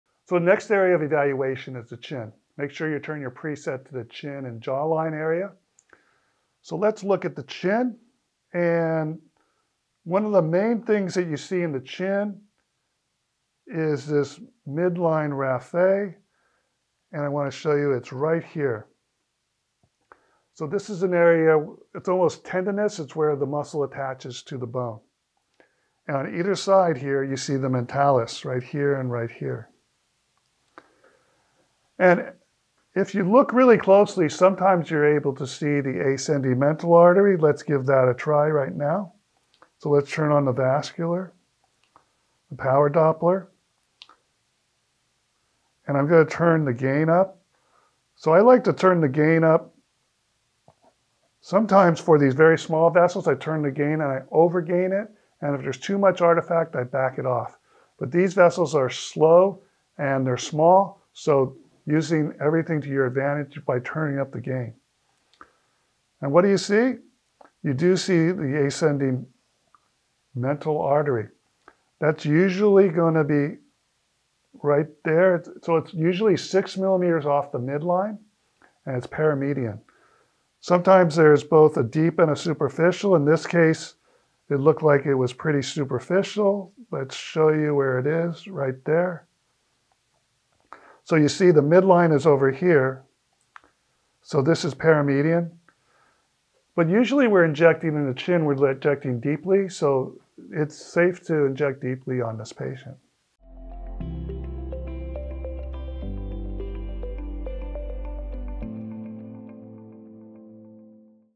Ultrasound of the Chin
In this video Dr. Weiner describes the ultrasound appearance of the chin, and how to image the ascending mental artery slightly lateral to the midline.